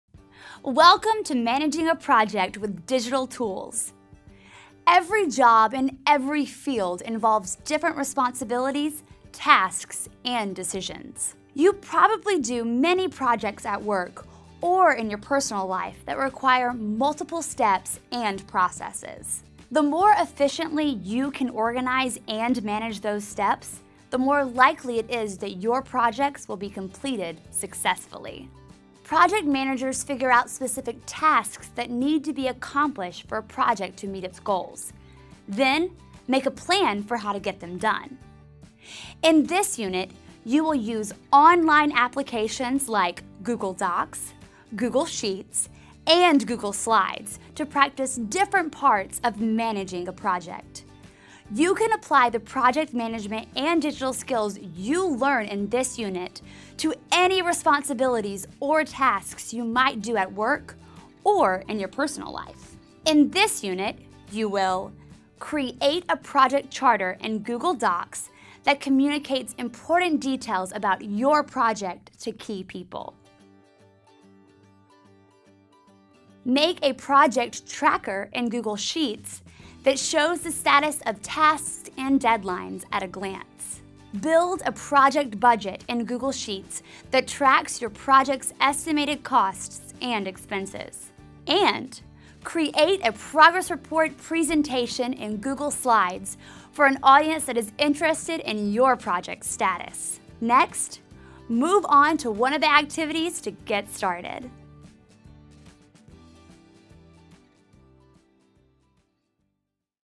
Introduction
Manage a Project with Digital Tools guides you through fulfilling  the responsibilities of a project manager by using digital tools. In this video, you'll learn what you'll do in this lesson, and how the G Suite applications help you along the way.

0:00 - Use digital tools to complete tasks
0:29 - How do project managers manage tasks?
0:39 - Which applications will you use and what activities will you complete?